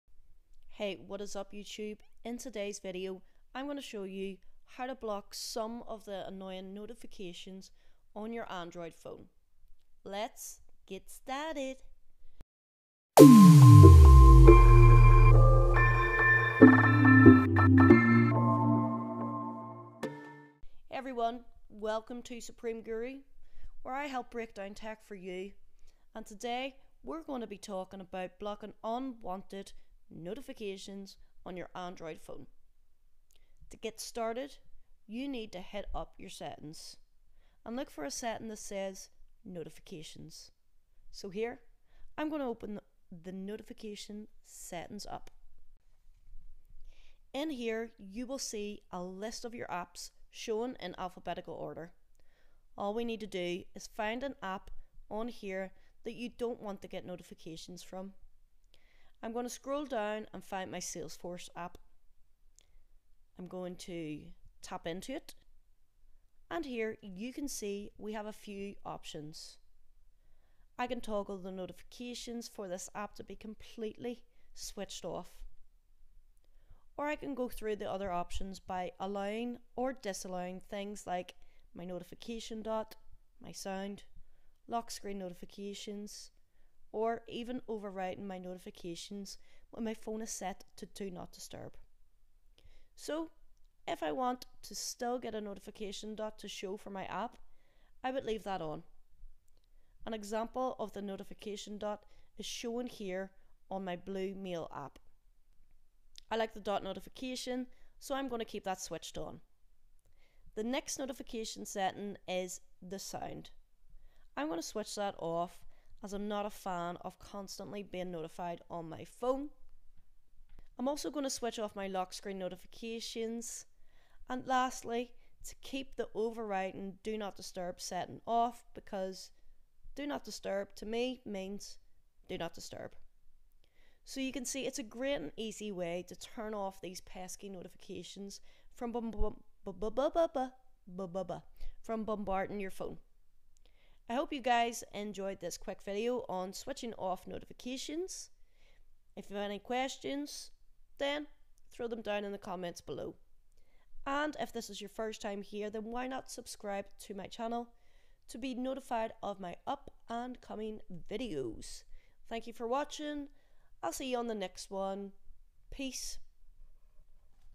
Silence the Noise! How to Control Notifications on Your Android (Easy Guide)
How To Disable Unwanted Notifications on Android

In this video I will show you how to turn off unwanted notifications on your android device.  There is nothing more annoying than receiving notifications you don't want to hear from on your phone.

I will show you step by step on how to disable unwanted notifications.

My Video Equipment:

Camera - Nikon D3300
Microphone - Samson Go Mic
Lighting - Abeststudio 135W Studio Light